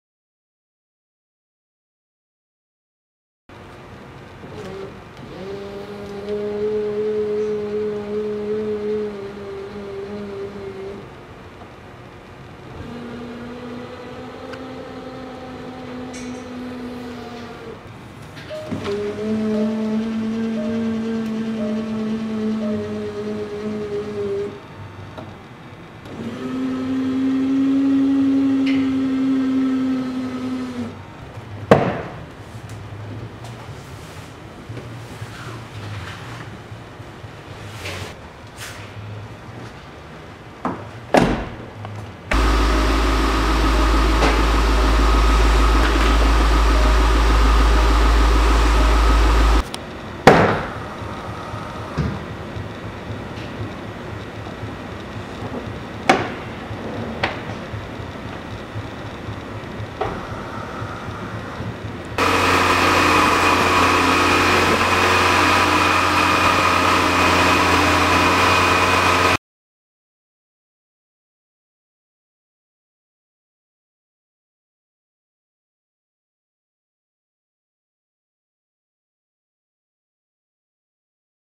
Alpine Armoring Armored Range Rover Long Wheelbase Autobiography Full VIP Bulletproof black on blac
Alpine Armoring shows us a brand new armored, bullet-proof Range Rover SUV.
Protection against high power rifles such as 7.62x39, 5.56x45, 7.62x51, M193, and M80 ball

Category
Autos & Vehicles
License